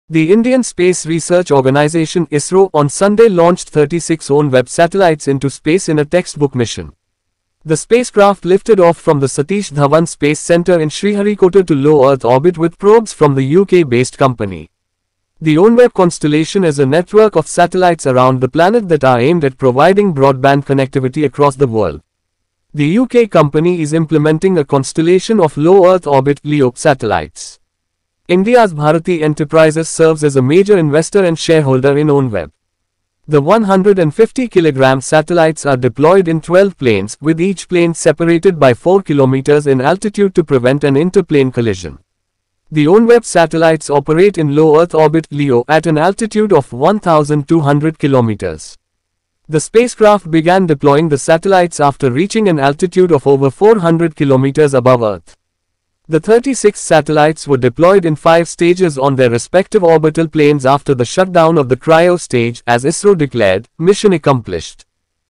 What Is OneWeb LEO Satellite and Its Purpose
OneWeb Satellite Broadband For All, ISRO Accomplished Placing 32 Satellites In Orbital Plane.